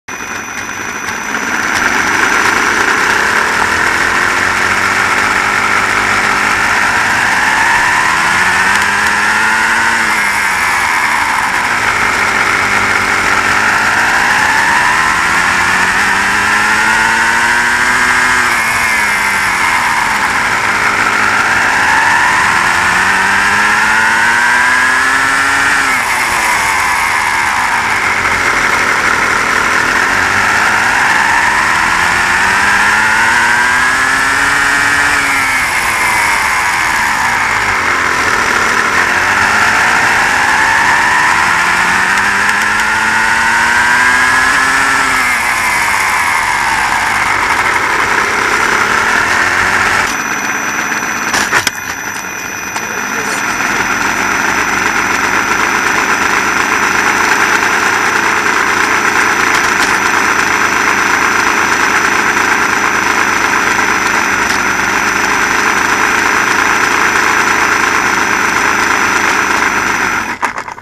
Lego Porsche 911 RSR Night Drive
In this video i drive my Lego Porsche 911 RSR At night with the new head light's i've installed and, i place the camera in the interior view and, so you can also hear the sound of, the car really well. So hope you enjoy the video.